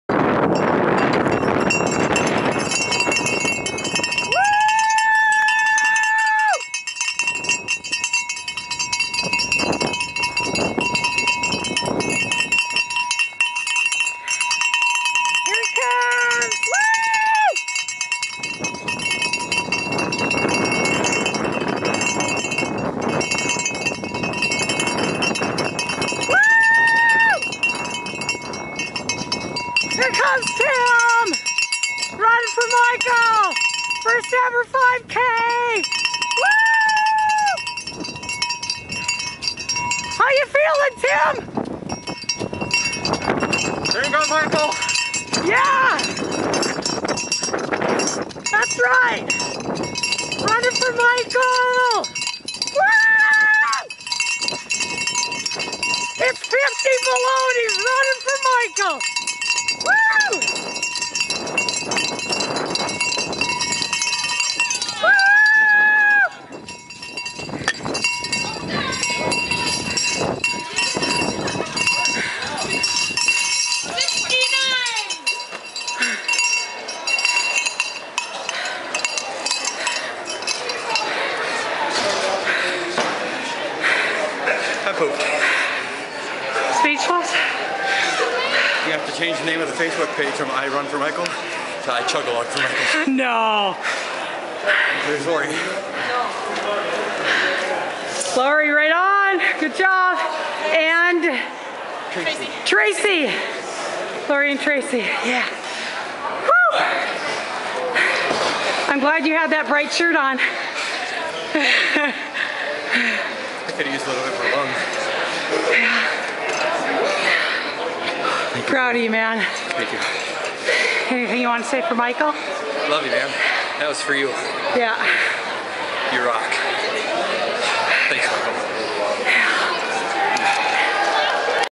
Sunshine Fest 2013 Tim Boyle 5K
Sunshine Fest in Grand Forks, Jan. 26, 2013. Tim ran his first 5K for Michael Wasserman who has downs.